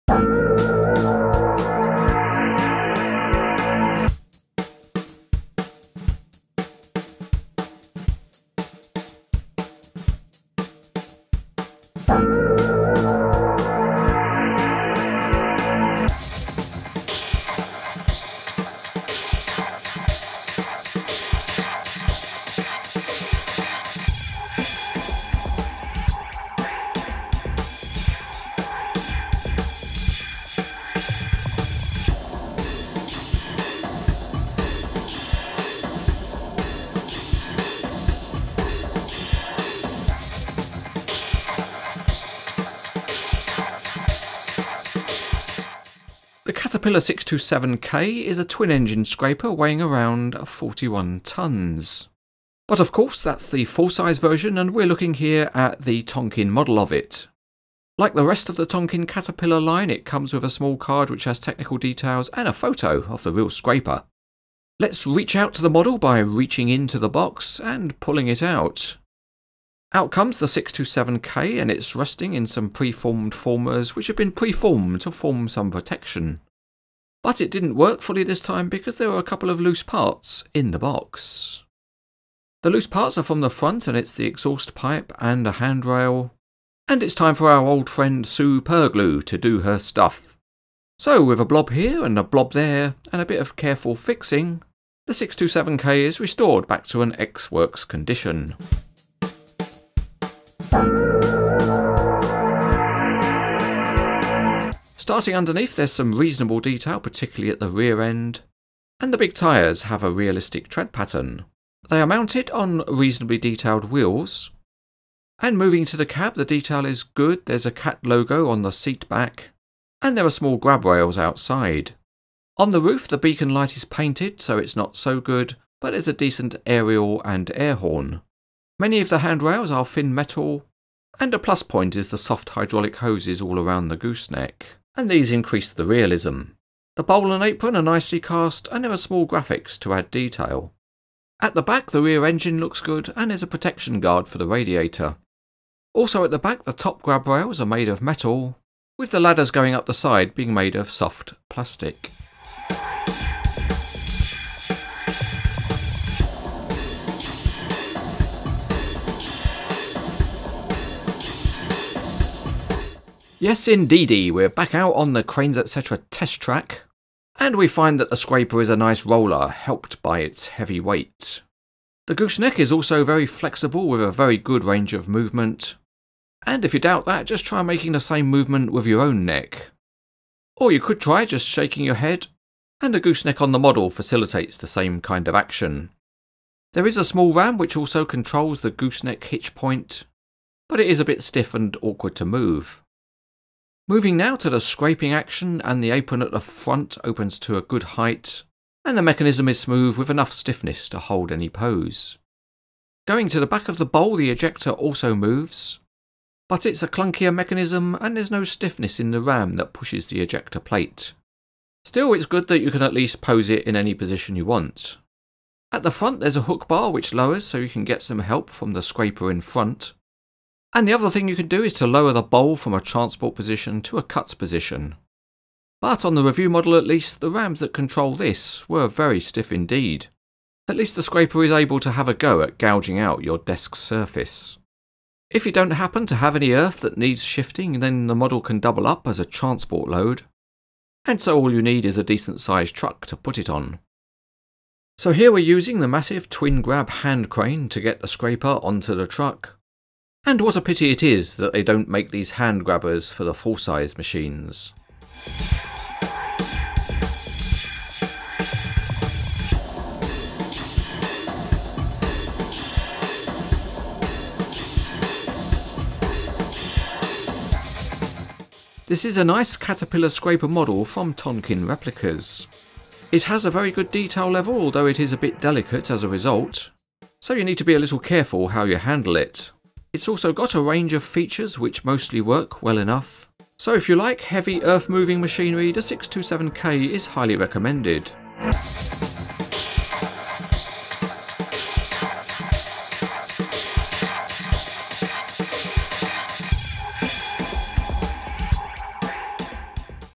Tonkin Caterpillar 627K Wheel Tractor Scraper by Cranes Etc TV
This is the Cranes Etc TV review of Tonkin Replica's 1/50 scale model of the Caterpillar 627K Wheel Tractor Scraper.  The model number is 80002.  The full review is on the Cranes Etc